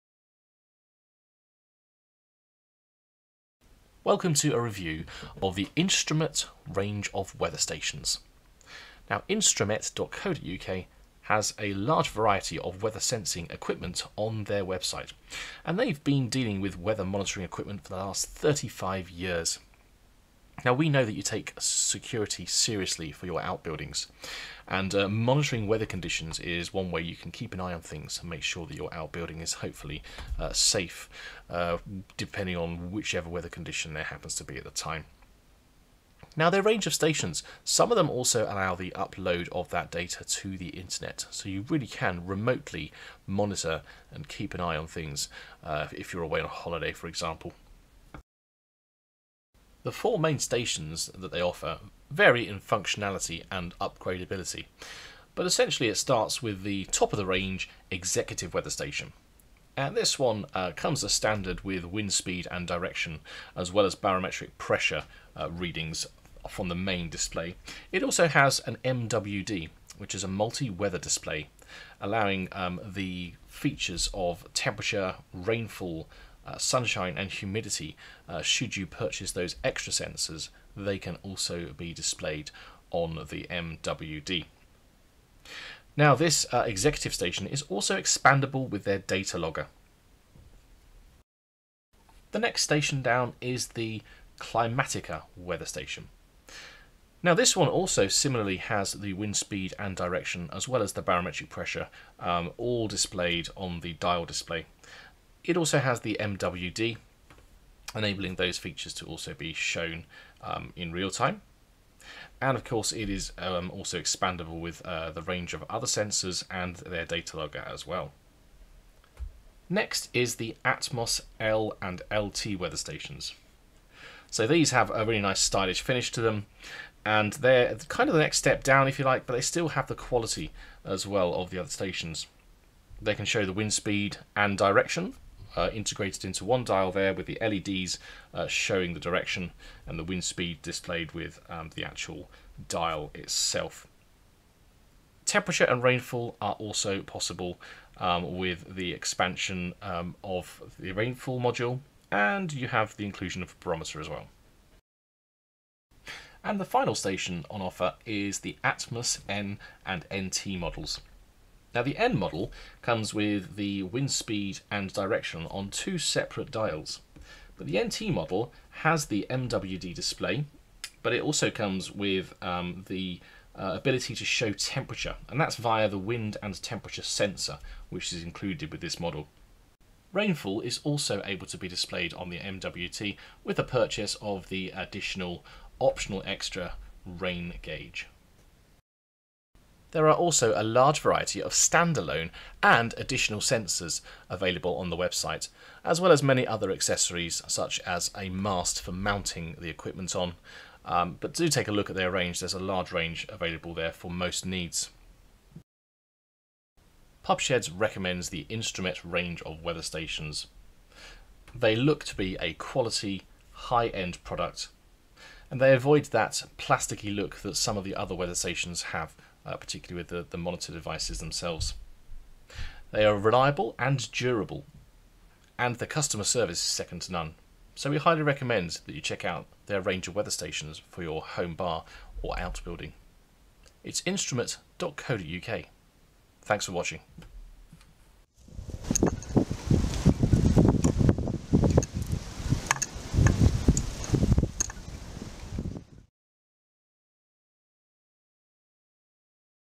**REVIEW** Instromet Weather Instruments - Pub Sheds review
We take a look at the range of weather stations and sensors available from Instromet.co.uk